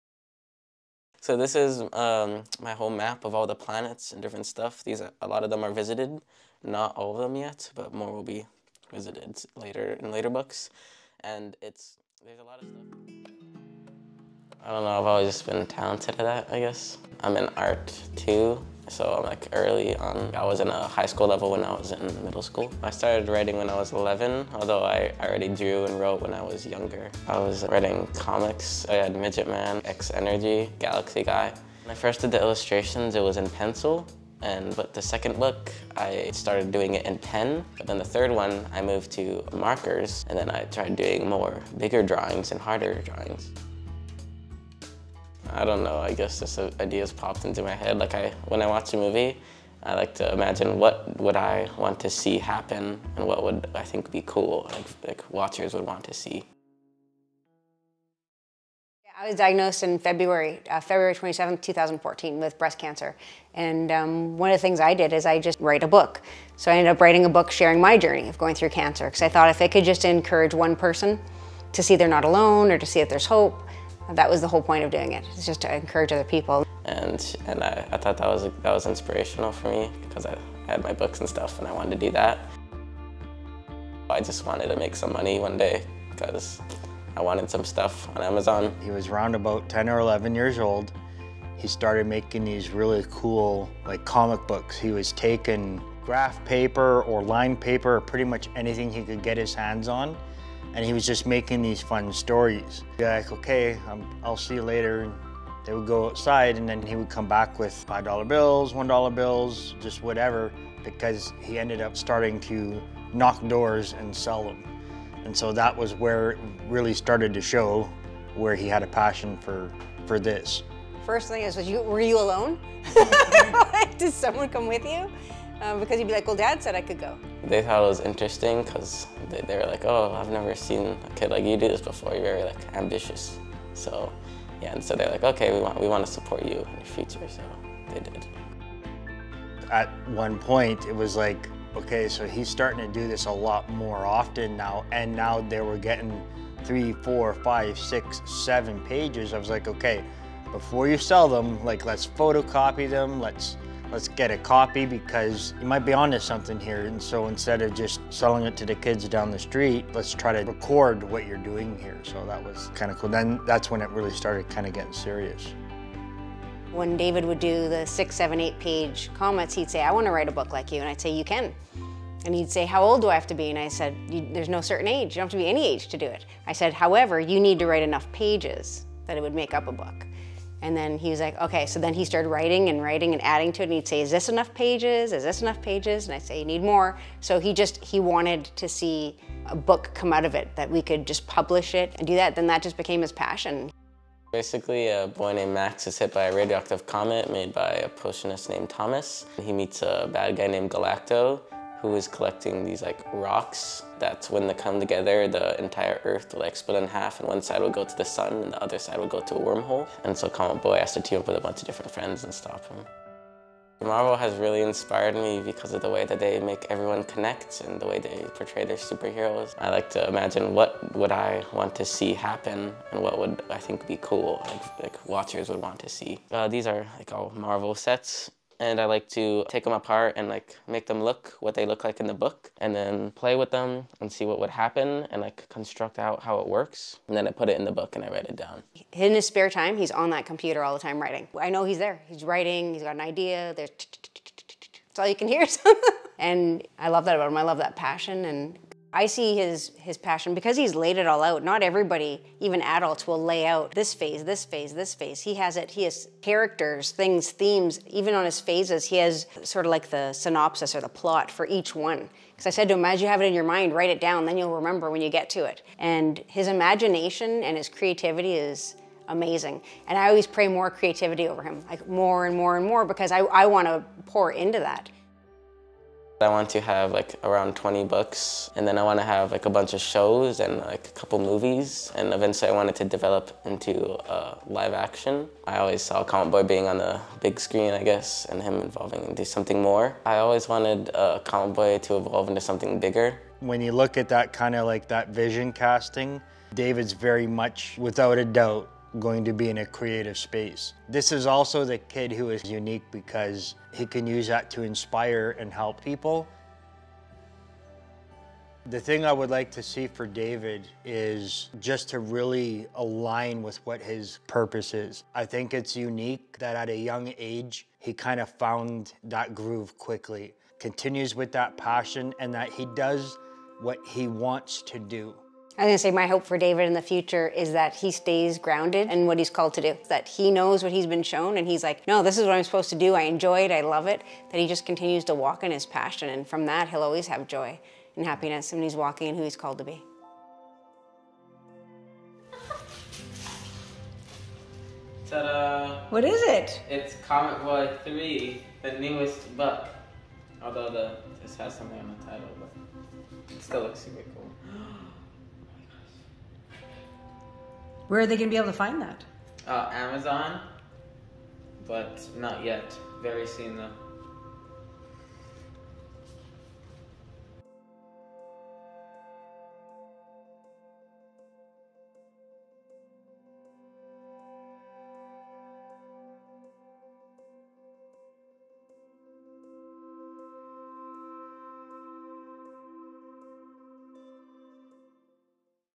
Comet Boy - Short Documentary // Spring 2023 // Cypress Woods High School
David Perzan, Cy Woods High School student, is a published author of 3 books with more to come. His story, Comet Boy, is about Max, an ordinary boy, who gets hit by a comet that is different. he is now a superhero fighting bad guys and saving the planet. Many bad guys come, but Comet Boy reigns! He is faced with challenges and one tough one that could end the Universe. The book can be found on Amazon for purchase...

This documentary shows David's great accomplishment at a very young age.

Edited by AVP 1 Student: Travis Westbrook
Cinematographers: Misty Mutters, Ethan Mough, Travis Westbrook